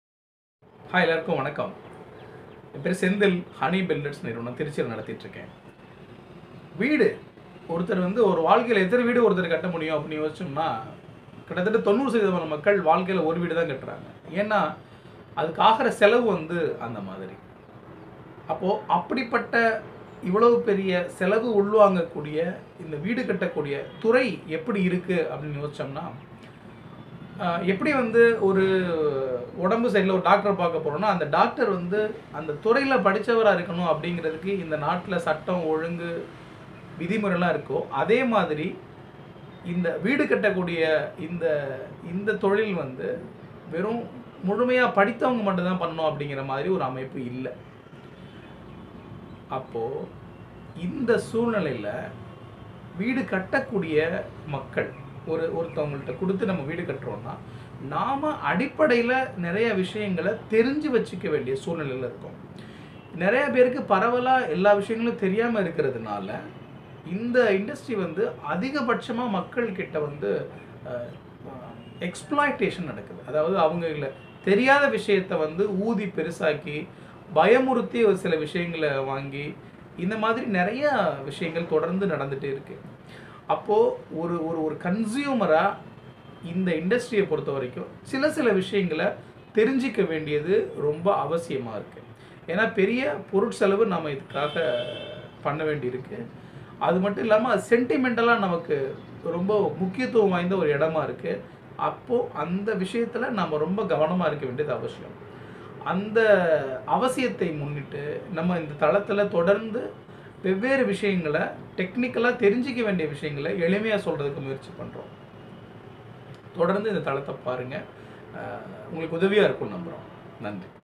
இன்றைய கட்டுமானத்துறை எப்படி இருக்கிறது? வீடு கட்டுபவர்கள் எதற்கு தயாராக வேண்டும் ?
இன்றைய கட்டுமானத் துறையின் நிலையைப் பற்றியும், வீடு கட்டுபவர்கள் எதற்கெல்லாம் தயாராக இருக்க வேண்டும் என்பதைப் பற்றியும் இந்த பதிவில் பேசலாம்.